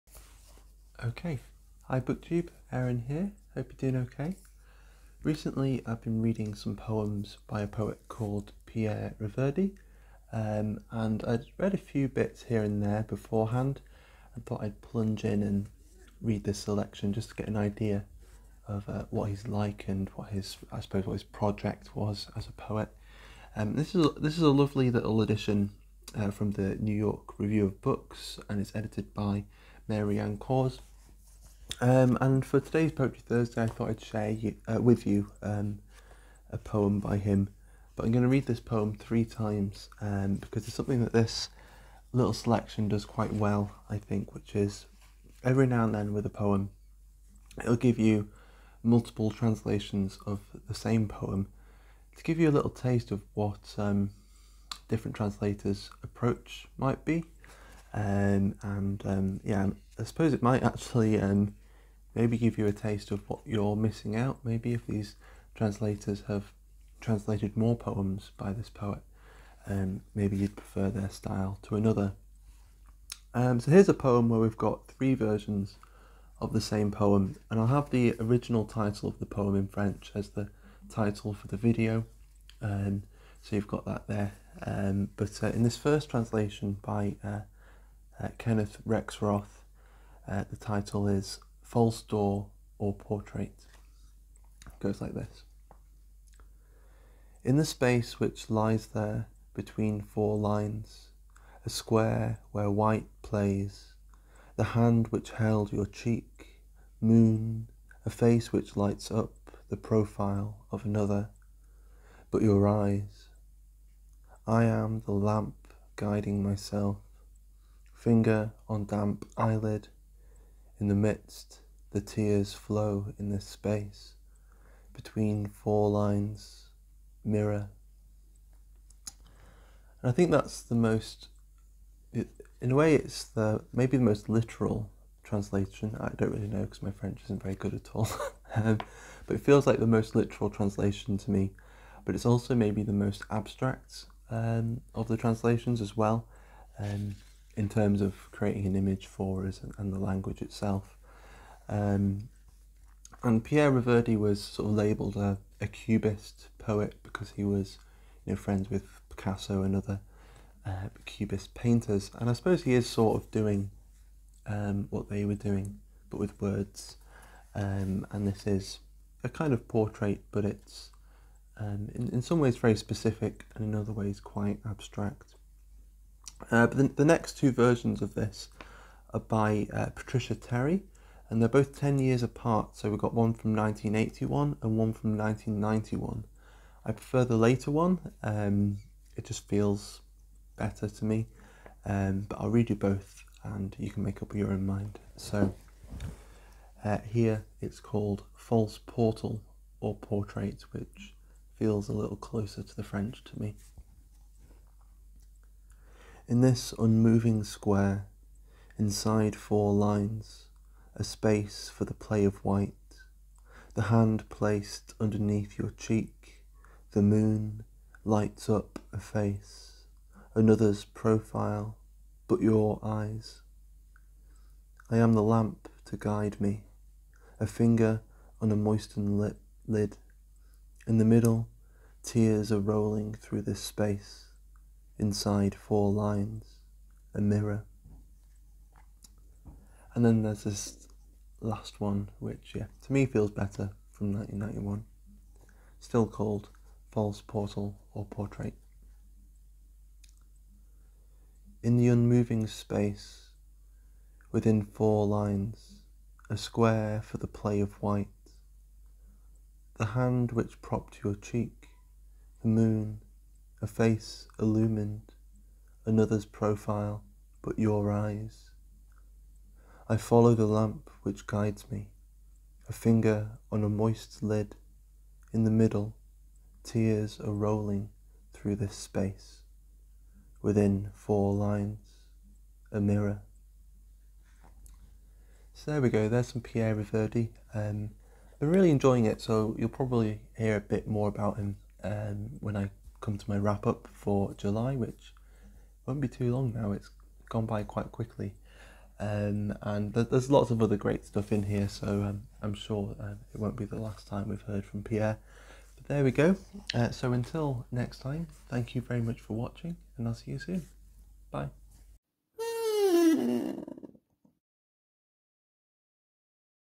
Poetry Thursday: "Fausse Porte ou Portrait" by Pierre Reverdy
In which I read the same poem by Pierre Reverdy three times -- and no, the record isn't broken.

Voxer: afacer916